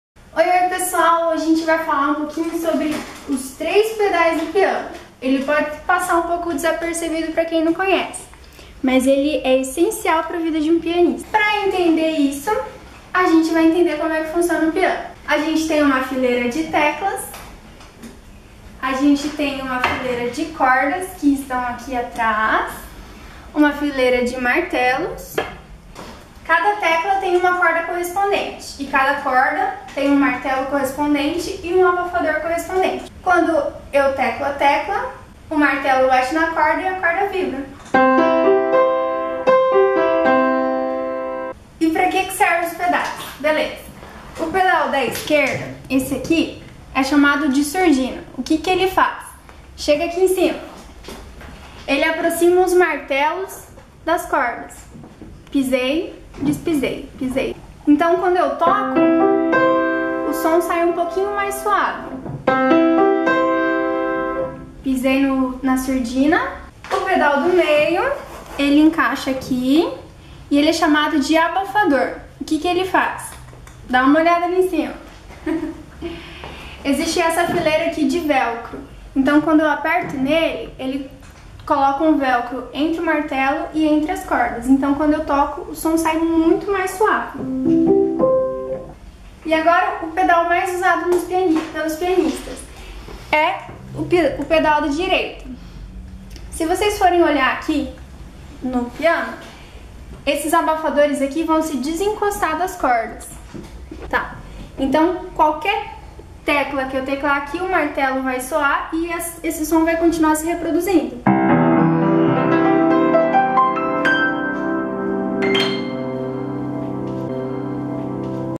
Como funciona os 3 pedais de um piano?
Física do cotidiano em 2 minutos - Vídeo produzido pela licencianda Shaini di Berardino Dittberner para a atividade de edição e produção de vídeos didáticos na disciplina de Estágio Supervisionado em Física III no curso de Licenciatura em Física da Universidade Estadual de Maringá no ano de 2016.